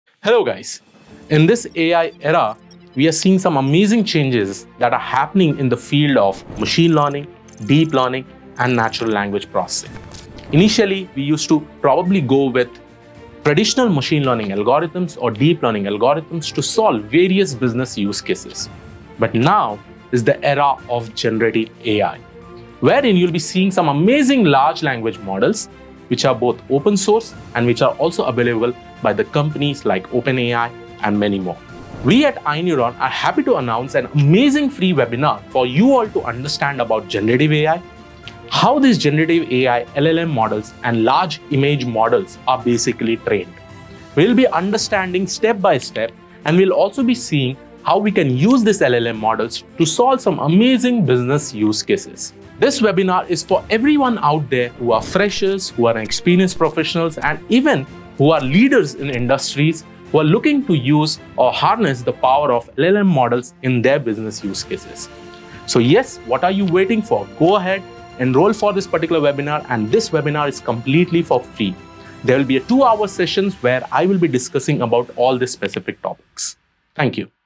Webinar - Generative AI Revolution : The Future | 2024
Don't Miss This Game Changing Webinar on "Generative AI Revolution : The Future | 2024"  by @krishnaik06 Sir

Date : 22 Dec 2023 ( Friday ) : Time : 6PM - 8PM